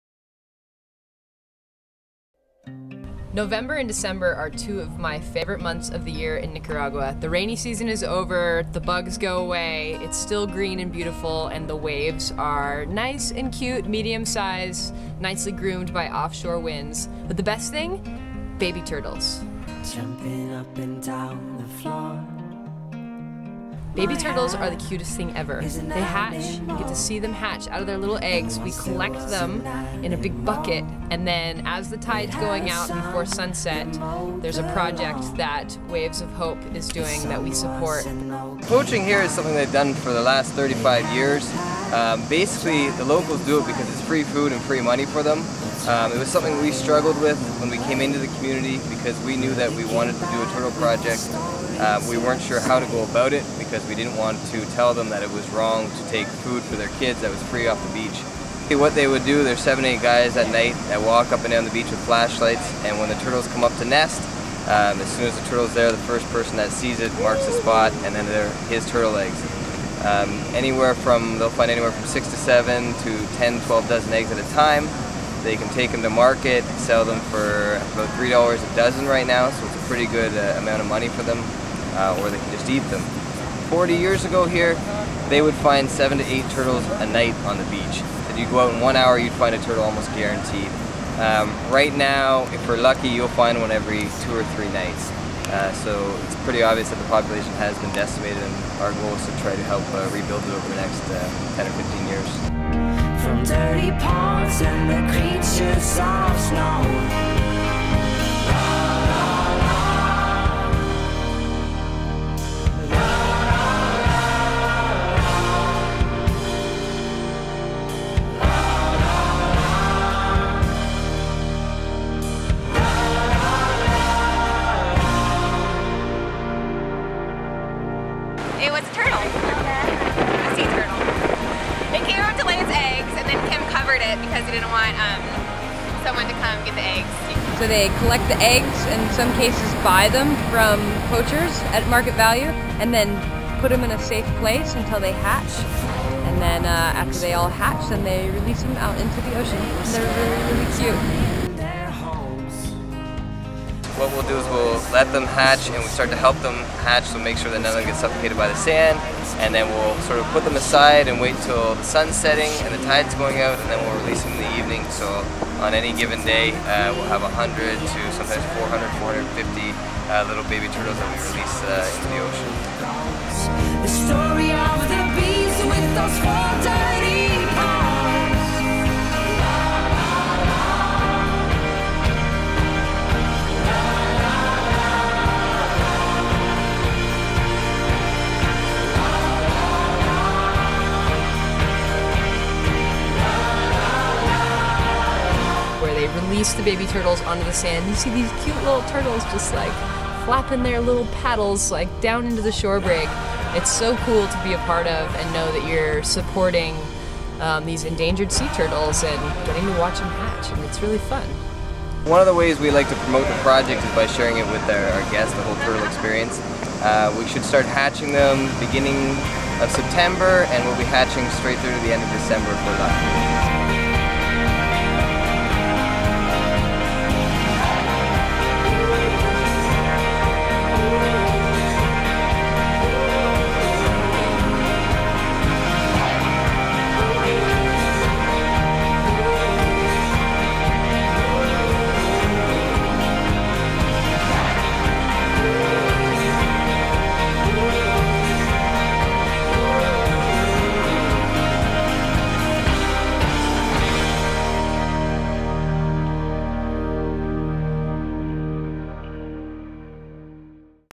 Surf With Baby Turtles in Nicaragua
November and December are two of my favorite months of the year in Nicaragua. The rainy season is over, everything is green and beautiful, the waves are cute and groomed by offshore winds, but the best part might be baby turtles! Surf With Amigas is partnered with El Coco Loco Resort and Waves of Hope on their many projects including a sea turtle conservation project. Watch the video to learn more and visit in November and December to watch baby turtles released into the ocean at sunset.